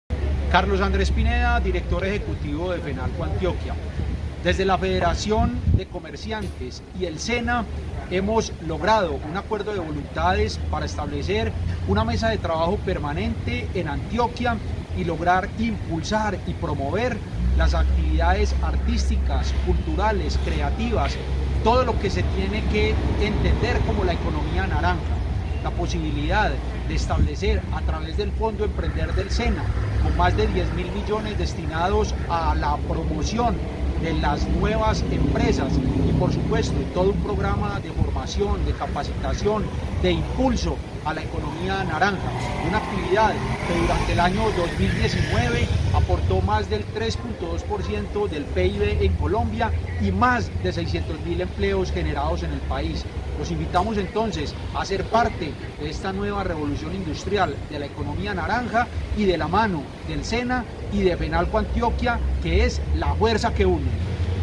SENA y Fenalco Antioquia se unen para fortalecer la Economía Naranja en la región
Promover y fomentar las industrias creativas y culturales, que integran la Economía Naranja, así como sus políticas, la importancia e incentivos, se convirtió en motivación para que el SENA y Fenalco Antioquia sellaran un acuerdo de voluntades para la creación de una mesa de trabajo para empoderar a las empresas que integran este sector y aprovechen todos sus beneficios.